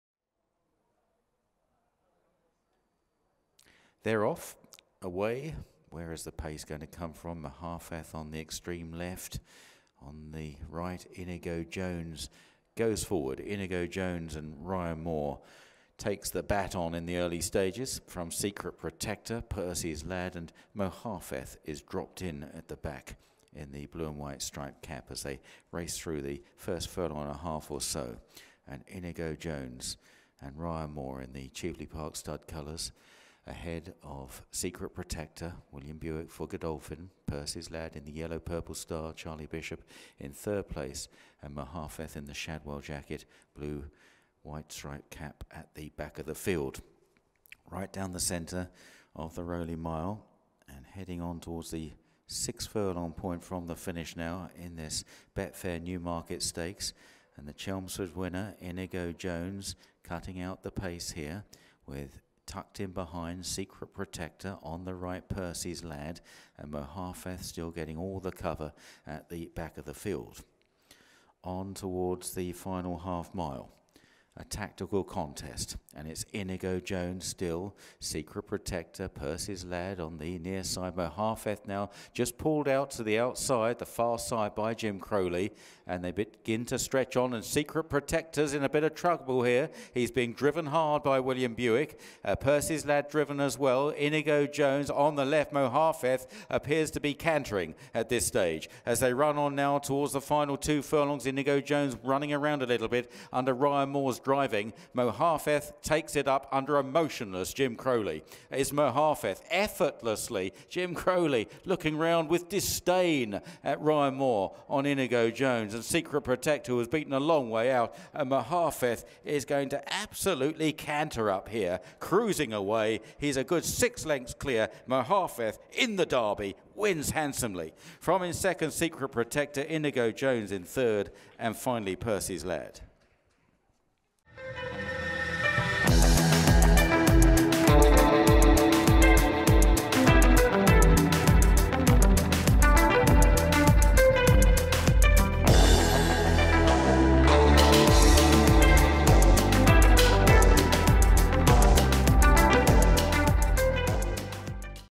MOHAAFETH ROARS into the Derby reckoning with this stylish success in the Betfair Newmarket Stakes
Jim Crowley moves his mount into a challenging position going into The Dip and he simply breezes clear to win unextended by five lengths. Can he make an impact in the Cazoo Derby at Epsom in June?